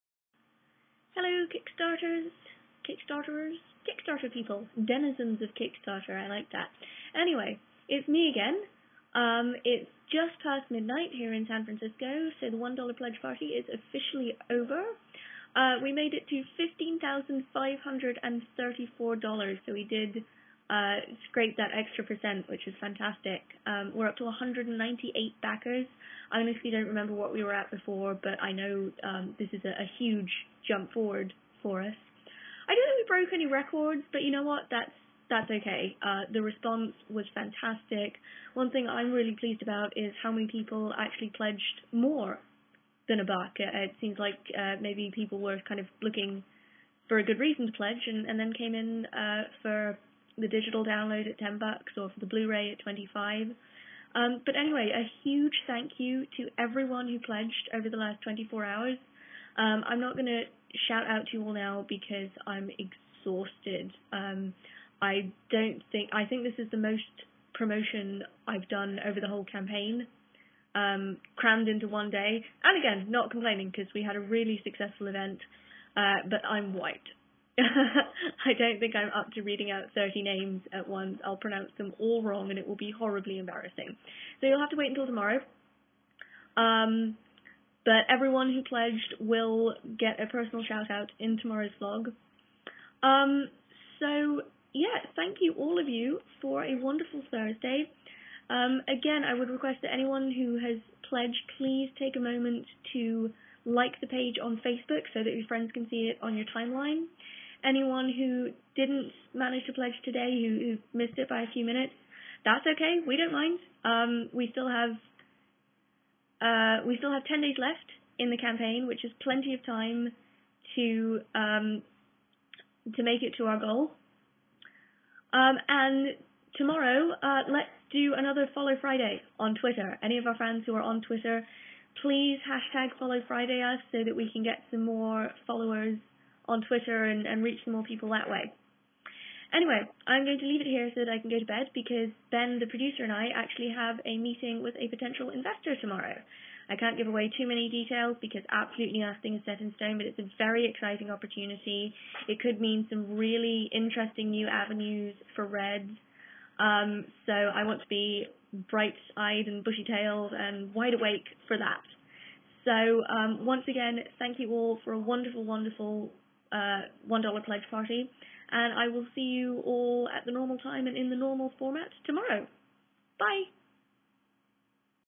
Day 31 (part 5) - Dani's Kickstarter Vlog
Day 31 (part 5) of Dani's Kickstarter vlog, in which she is tired, but altogether rather pleased. Visit kck.st/JHs9kB to support Films by Neptune's "Red"!